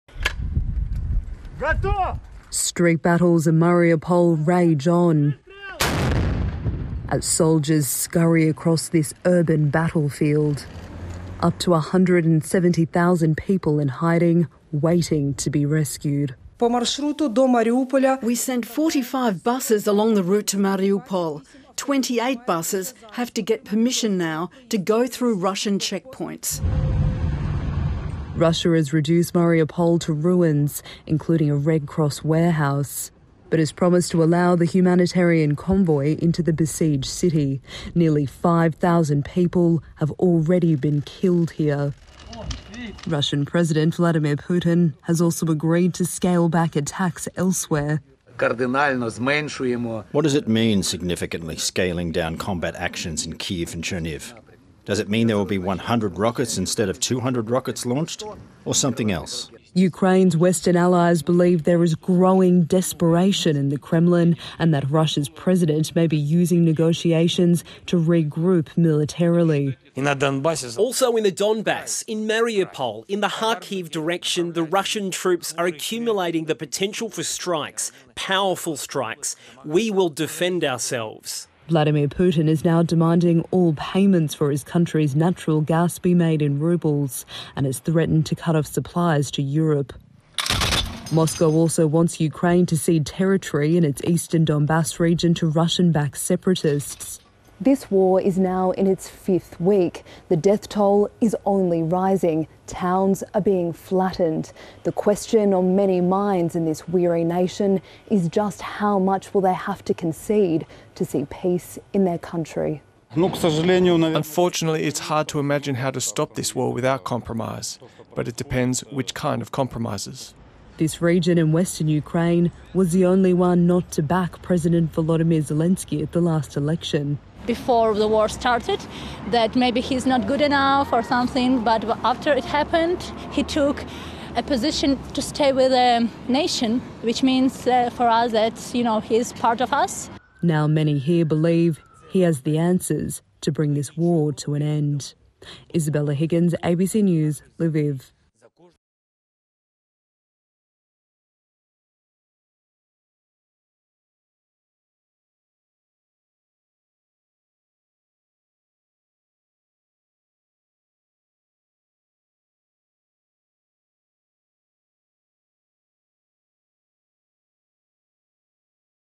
Ukraine warned of more assaults from regrouping and reinforcing Russian forces | ABC News
NATO says the Kremlin's change in tactics isn't a retreat and it warns that the invading forces are regrouping and reinforcing.